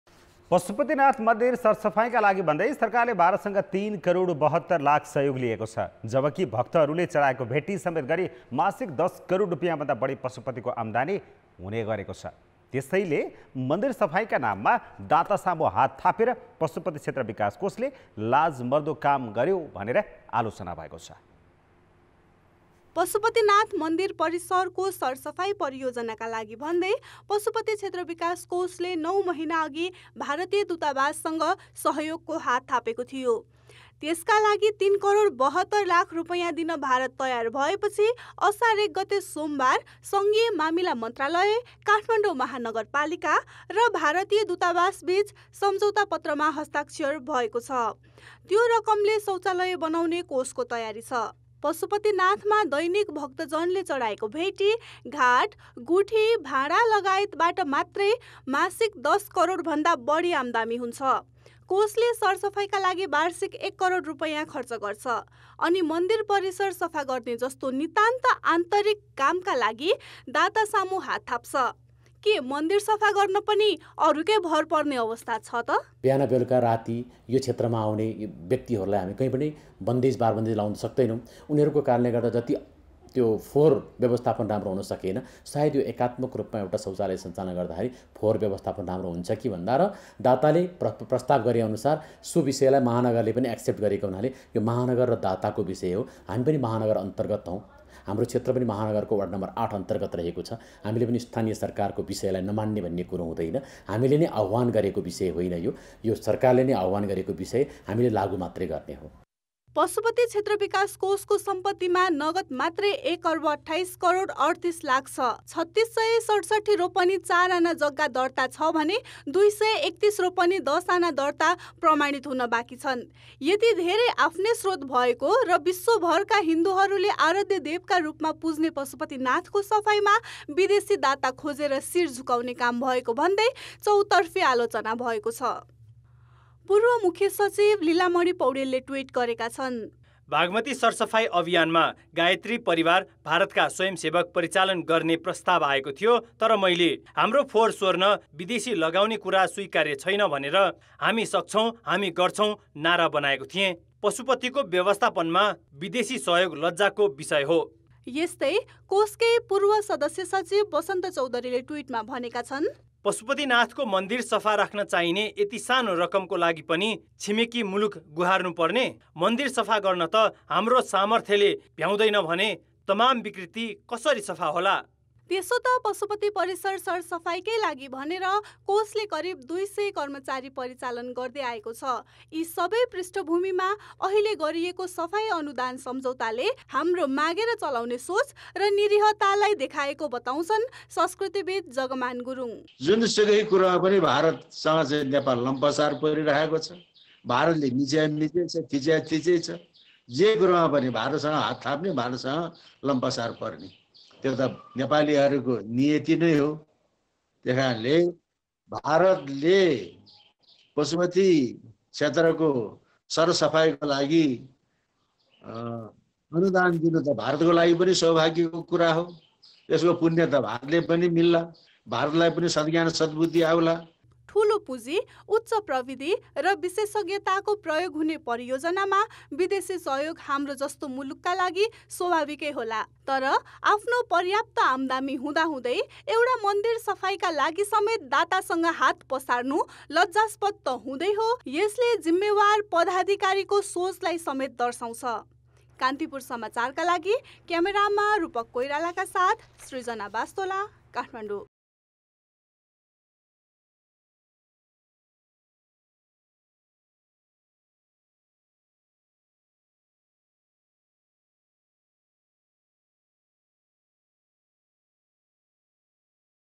पशुपतिनाथको सफाइमा विदेशी दाता खोजेर शीर झुकाउने काम भएको भन्दै आलोचना
पशुपतिनाथ मन्दिर सरसफाइका लागि भन्दै सरकारले भारतसँग तीन करोड ७२ लाख सहयोग लिएको छ । जब कि, भक्तहरुले चढाएको भेटी समेत गरी मासिक दश करोड रुपैयाँभन्दा बढी पशुपतिको आफ्नै आम्दानी हुन्छ । त्यसैले मन्दिर सफाइका नाममा दातासामु हात थापेर पशुपति क्षेत्र विकास कोषले लाजमर्दो काम गरेको भने आलोचना भएको छ ।